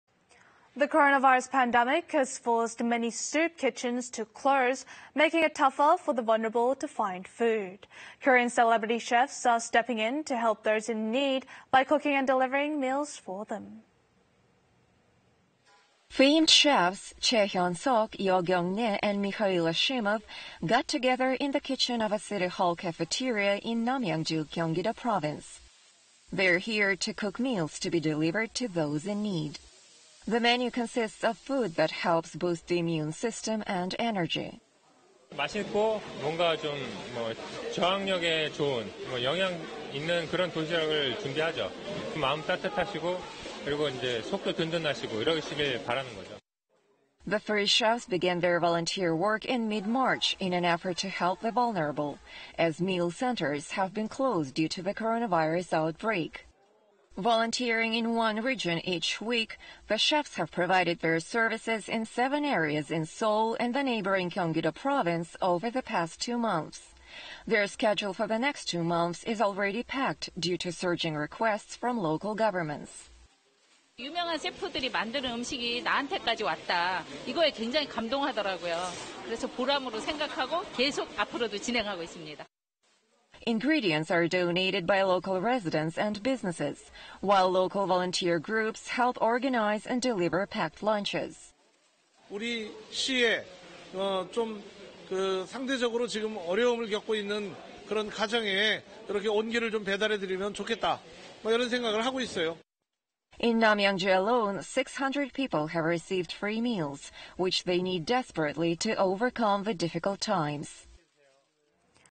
CELEBRITY CHEFS HELPS OUT THOSE IN NEED / KBS뉴스(News)
[Anchor Lead]
The coronavirus pandemic has forced many soup kitchens to close, making it tougher for the vulnerable to find food. Korean celebrity chefs are stepping in to help those in need by cooking and delivering meals for them.
[Pkg]
Famed chefs -- Choi Hyun-seok, Yeo Kyung-rae and Mihal Ashminov - got together in the kitchen of a city hall cafeteria in Namyangju, Gyeonggido Province. They are here to cook meals to be delivered to those in need. The menu consists of food that helps boost the immune system and energy.
[Soundbite] CHOI HYUN-SEOK(CHEF) : "We prepare delicious and nutritious meals that are good for the immune system. We want to give hope and strength to those in need."
The three chefs began their volunteer work in mid-March in an effort to help the vulnerable, as meal centers have been closed due to the coronavirus outbreak. Volunteering in one region each week, the chefs have provided their services in seven areas in Seoul and the neighboring Gyeonggi-do Province over the past two months. Their schedule for the next two months is already packed due to surging requests from local governments.
[Soundbite] YOO HYUN-SOOK(SHARING CULTURE & ARTS ASSOCIATION) : "When people receive meals, they are moved to know that the food was cooked for them by famous chefs. We will continue this program, as it's very rewarding."
Ingredients are donated by local residents and businesses, while local volunteer groups help organize and deliver packed lunches.
[Soundbite] CHO KWANG-HAN(NAMYANGJU MAYOR) : "I hope that this volunteer program will deliver hope and warmth to those in need in our city."
In Namyangju alone, 600 people have received free meals, which they need desperately to overcome the difficult times.